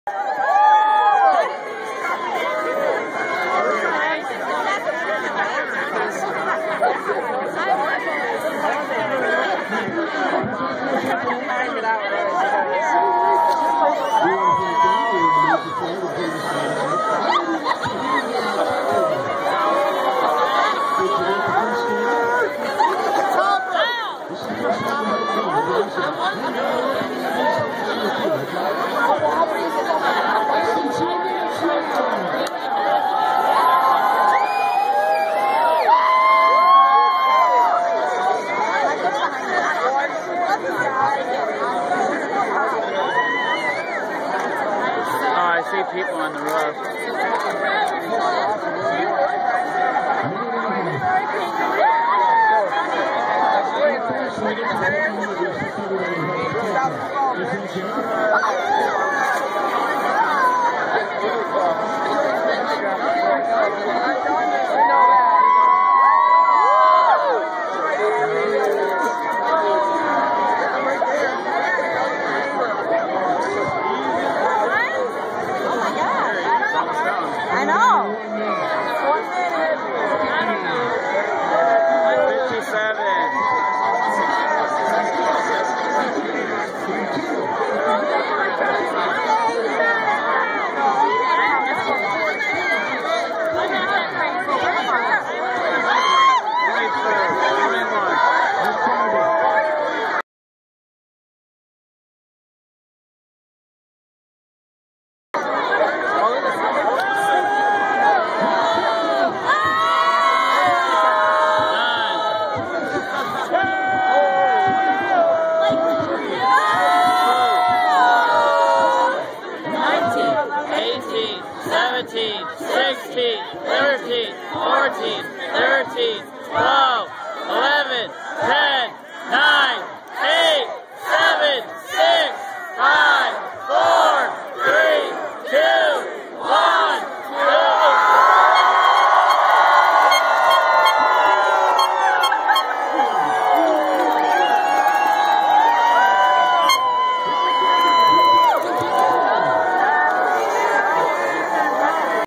Mcmorran New Year`s Eve Anchor Drop Celebration
The first year Celebration of the Anchor Drop at Downtown port huron by the Mcmorran arena Tower.